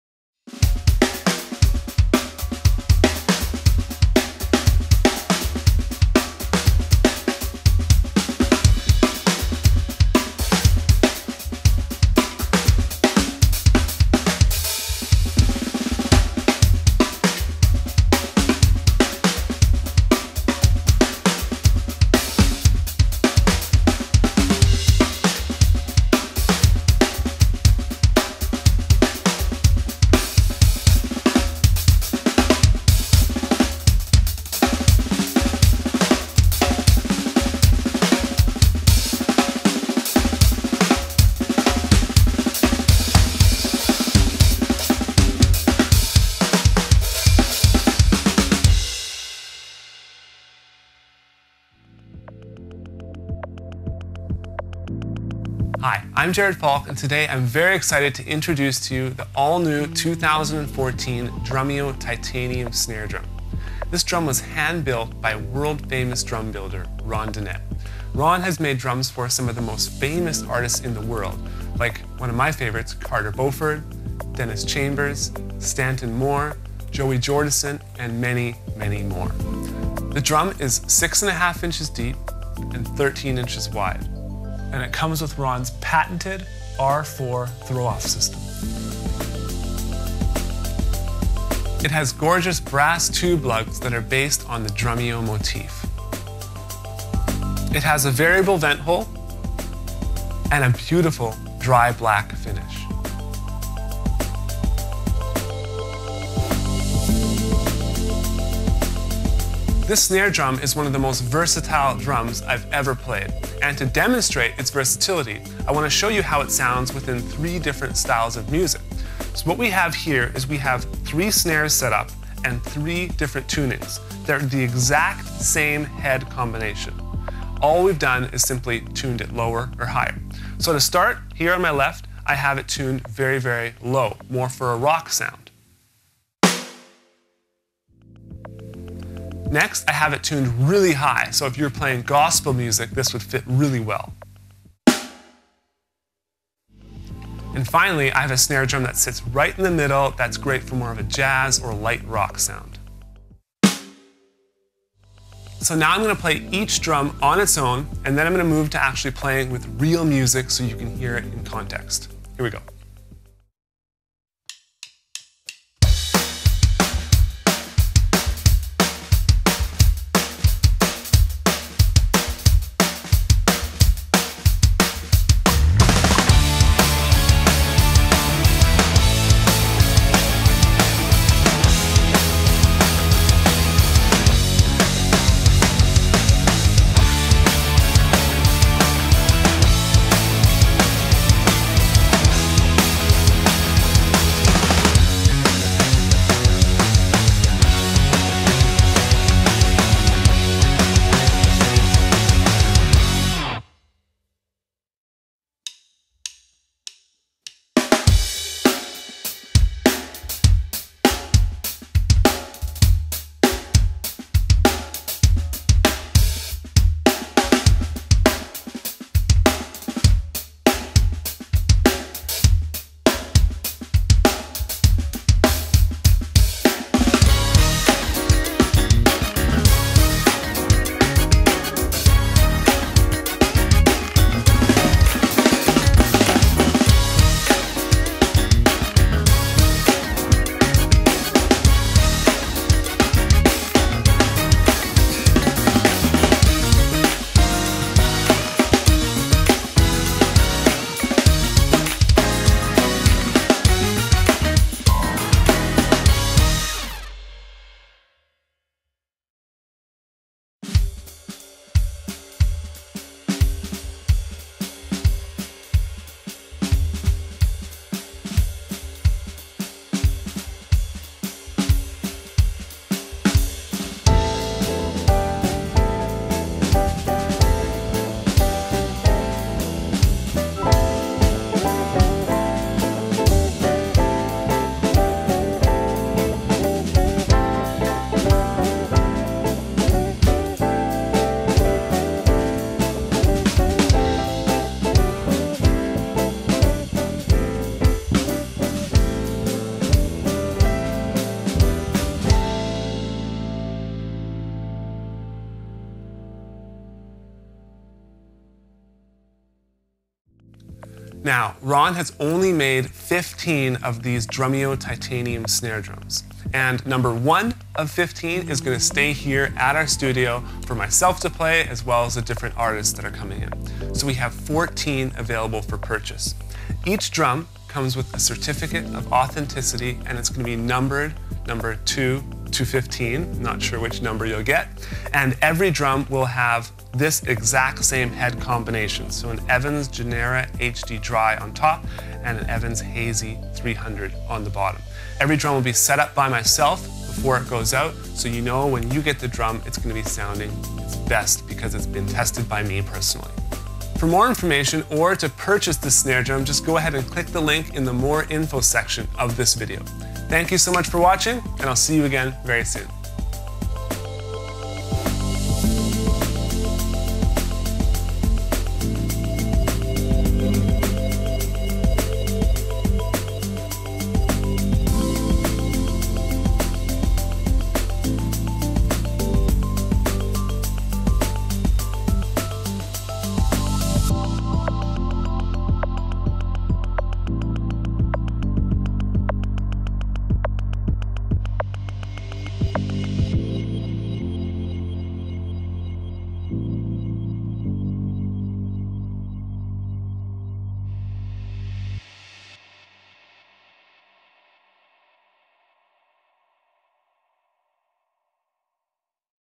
Drumeo Titanium Snare Drum
Stunningly Designed

Hand build by world renowned snare drum specialist Ronn Dunnett, featuring his patented R4 throw off system, premium Evans heads, gorgeous brass tube lugs, a variable vent hole, and a beautiful dry black finish.

Insanely Versatile
Clear and articulate, the 13” x 6.5“ titanium shell can be easily tuned for a wide range of musical styles.

Authentically Yours

Each snare comes with a certificate of authenticity, numbered between 2-15 - and will be personally set up and tested by Jared Falk, so you know it will sound its best the first time you sit down to play.